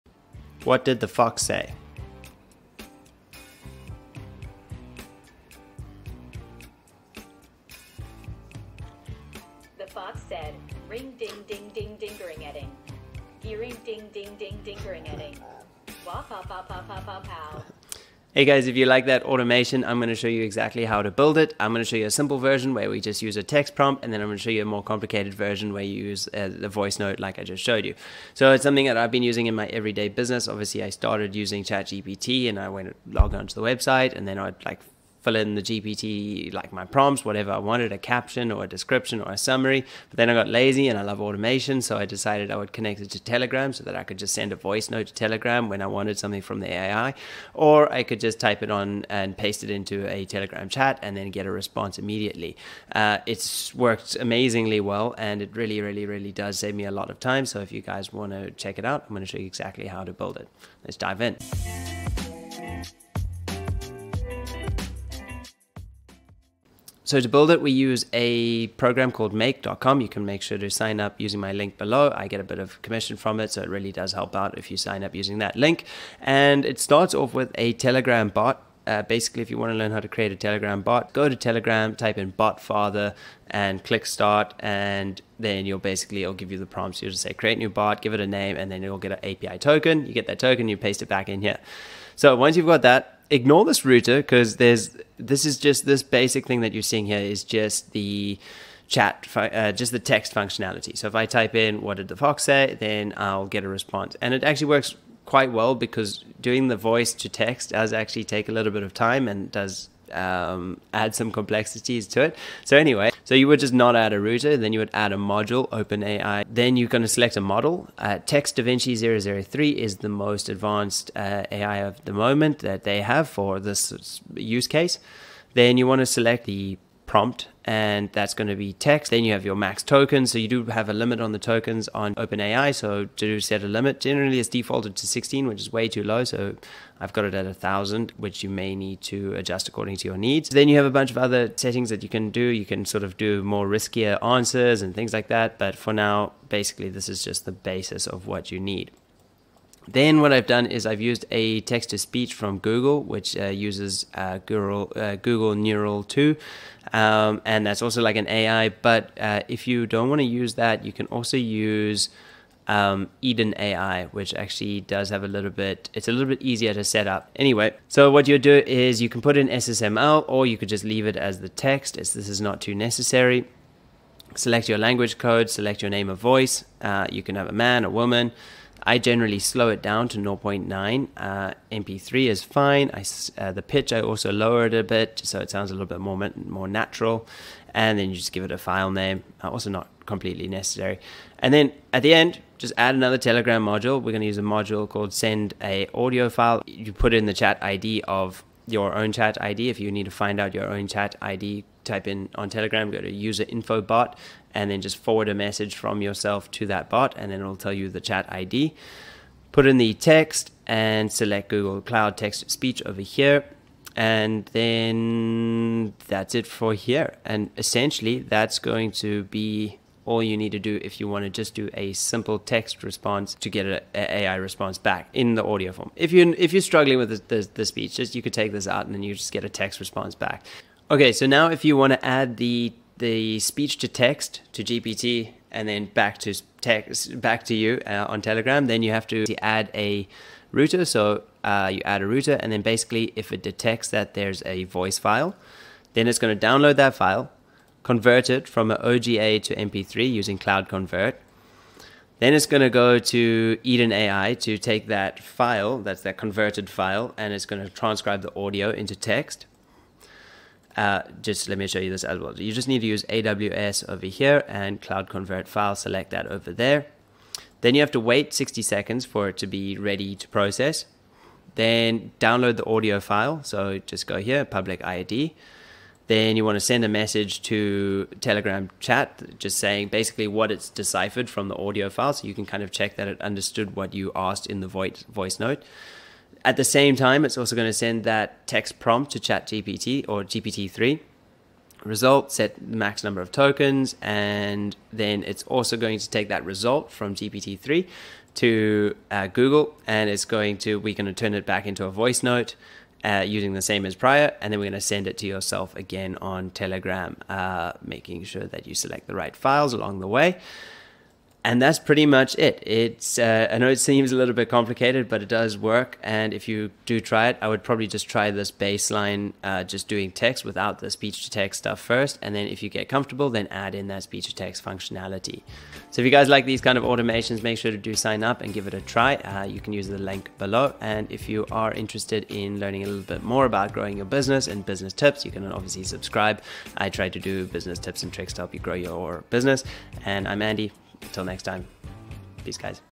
Turn ChatGPT into a Telegram Voice Assistant with Make.com (Full Build)
I have been using this integration for a while now. I connected telegram to OpenAI which is the foundation of chatgpt. Now that I have telegram connected to open AI I can easily send a prompt to gpt3 for a response. If I am busy I can also send a voice note and it will automatically transcribe it and send it to GPT3.

Get ahead in business with some of my favourite apps

Sign up for my email newsletter for more important and useful information